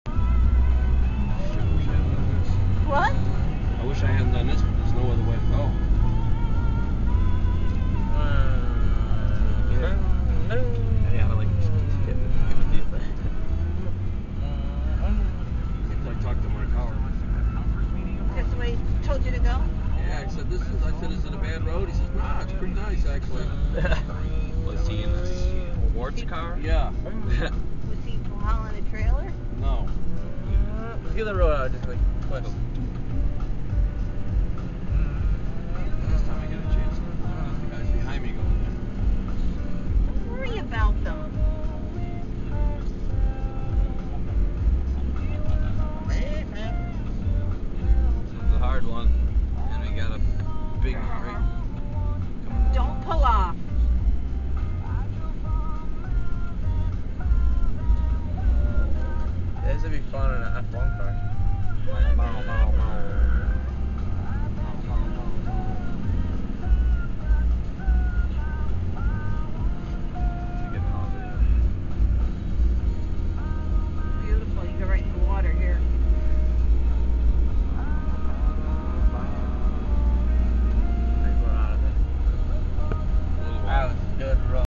Virginia mountains
haulin' the race car through the mountains in Virginia